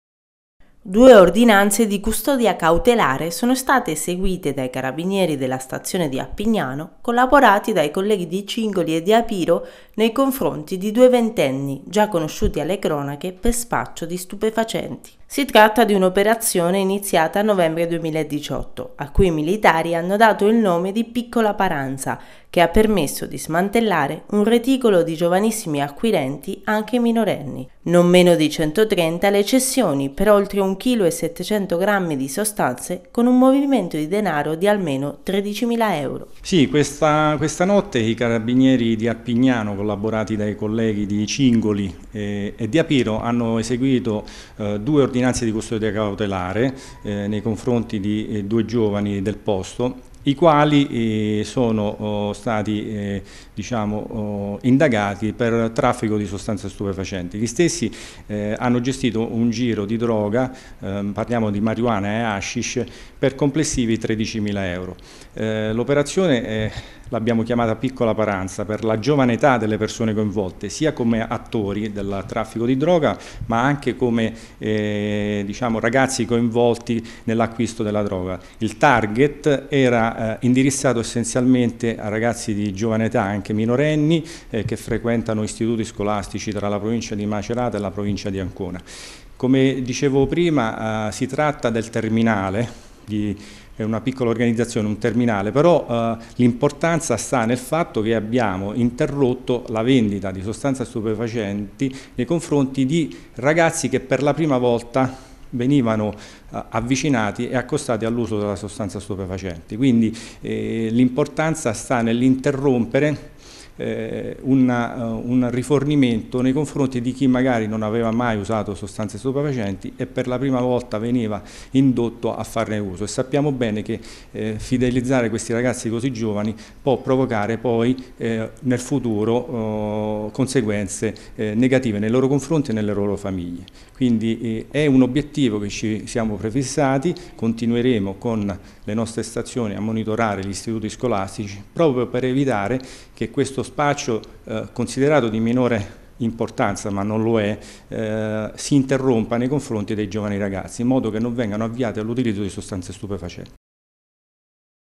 NEWS. I Carabinieri di Appignano arrestano due giovanissimi per spaccio
Due ordinanze di custodia cautelare per altrettanti giovanissimi di Cingoli. I Carabinieri di Appignano, Cingoli e Apiro smantellano un reticolo di acquirenti di sostanze stupefacenti anche minorenni.